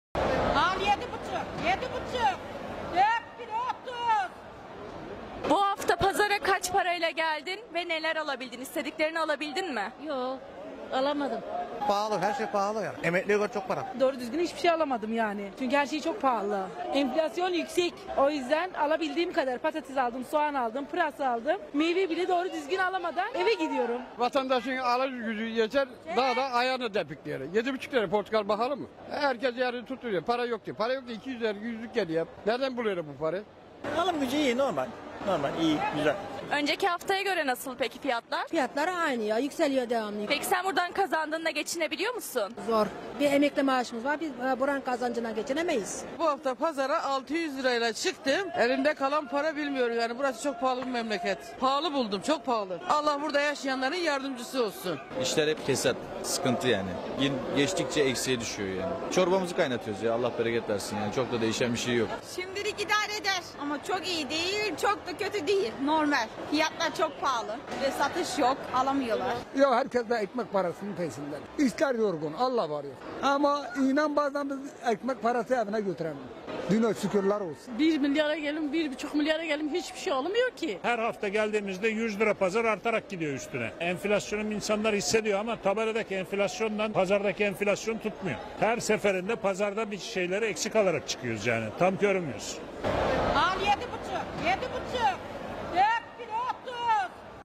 Her hafta mutfak alışverişi yapabiliyor musunuz?
Semt pazarlarında fiyatlar nasıl? Her hafta mutfak alışverişi yapabiliyor musunuz?